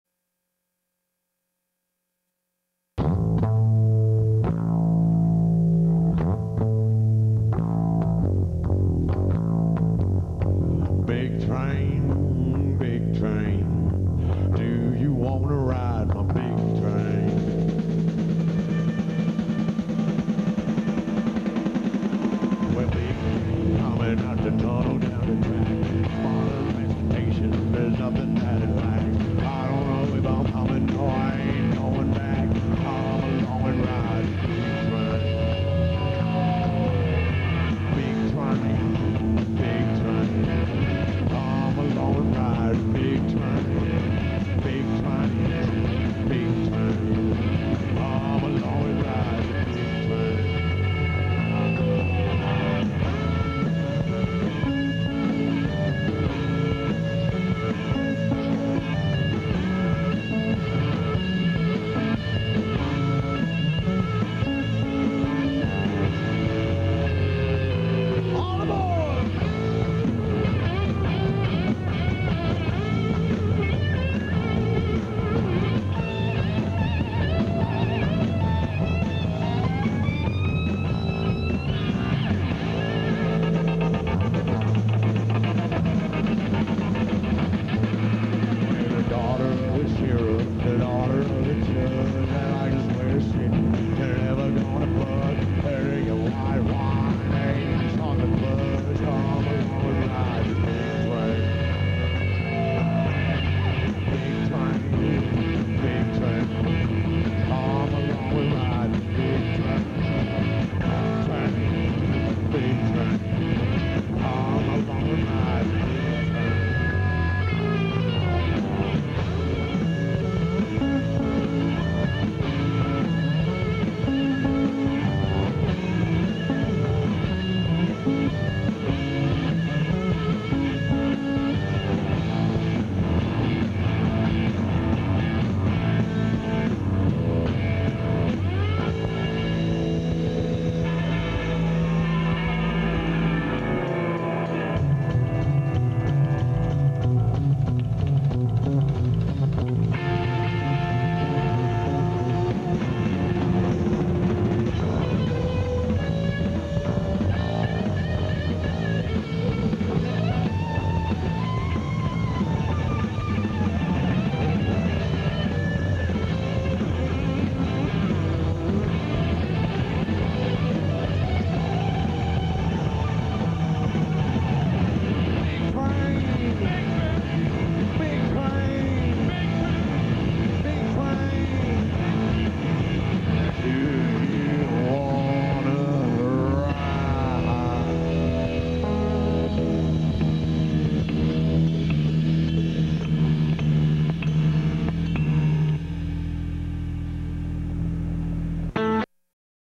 Mike Watt 4-30-95 TV performance
Mike Watt in a television studio performance recorded 4-30-95.